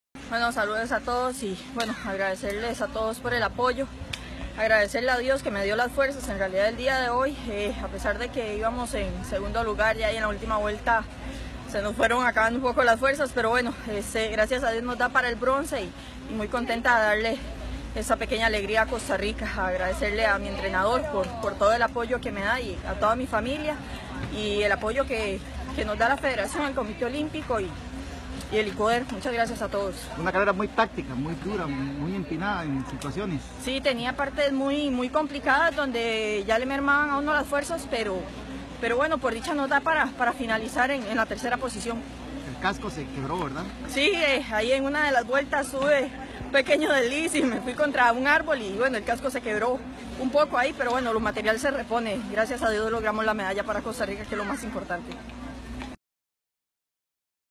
Milagro Mena aporta bronce en el ciclomontañismo regional
La costarricense Milagro Mena ganó este sábado la medalla de bronce en la prueba de ciclismo de montaña de los XXIII Juegos Deportivos Centroamericanos y del Caribe, disputada en la Pista Pelícanos de Puerto Colombia, en Barranquilla, Colombia.